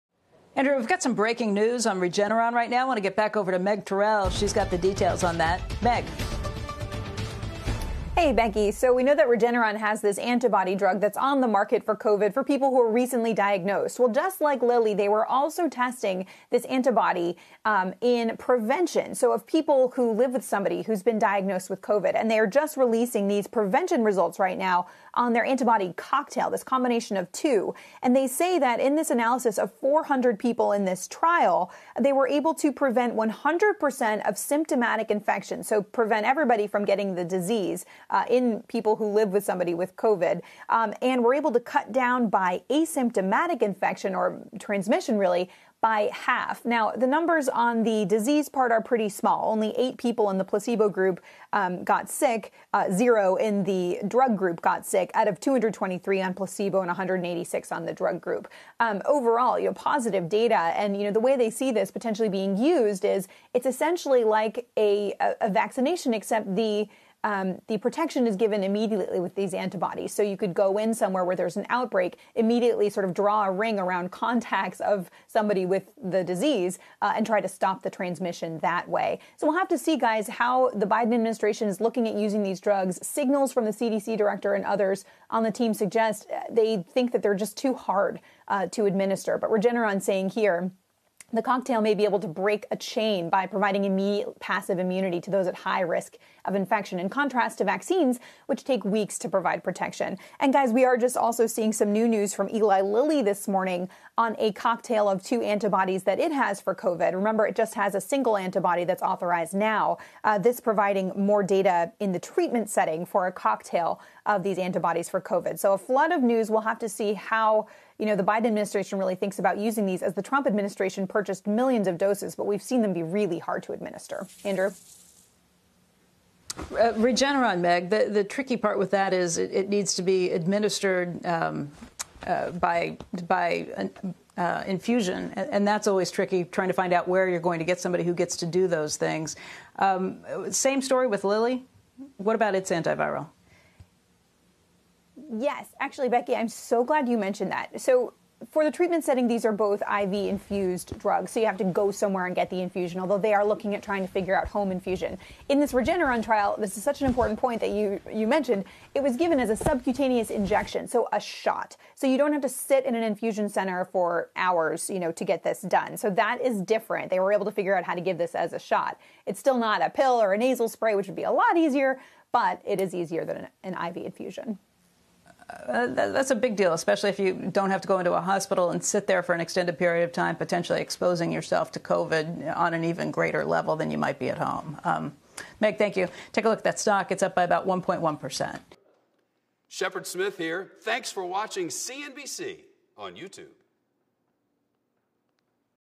Regeneron antibody drug shown to prevent symptomatic Covid-19
Regeneron reported Tuesday that its antibody cocktail for coronavirus has show a reduction in overall infections seen within the first week, with 100% prevention of symptomatic infections. CNBC's Meg Tirrell reports.

Regeneron Pharmaceuticals said on Tuesday its antibody cocktail was effective in preventing Covid-19 in people exposed to those infected with the new coronavirus, based on interim results from a late-stage study.

The two-antibody cocktail, REGEN-COV, caused a 100% reduction in symptomatic infection and roughly 50% lower overall rates of infection, based on an analysis of about 400 participants in the trial who had a household member with Covid-19.

Regeneron said it would discuss the interim results with U.S. health regulators to potentially expand the antibody cocktail’s current emergency use authorization (EUA). Full data from the trial is expected early in the second quarter

The U.S. Food and Drug Administration in November granted EUA to the antibody cocktail for the treatment of mild to moderate Covid-19 in adults and children.

The current trial tested REGEN-COV for use as a passive vaccine, which involves directly delivering virus-fighting antibodies into the body unlike traditional vaccines in which the receiver’s immune system is activated to develop its own antibodies.

REGEN-COV may be able to provide immediate passive immunity to those at high risk of infection, in contrast to active vaccines that take weeks to provide protection, the company said.

“These data using REGEN-COV as a passive vaccine suggest that it may both reduce transmission of the virus as well as reduce viral and disease burden in those who still get infected,” said George Yancopoulos, president and chief scientific officer of Regeneron.

The drugmaker also expects REGEN-COV to help people who respond poorly to vaccination.

During the trial, jointly run by Regeneron and the National Institute of Allergy and Infectious Diseases, one death and a Covid-19 related hospitalization were reported among those who received placebo, but there was no such incident in the treatment group, the company said.

REGEN-COV is also being studied in two late-stage hospitalized patient trials and a late-stage trial for the treatment of non-hospitalized patients.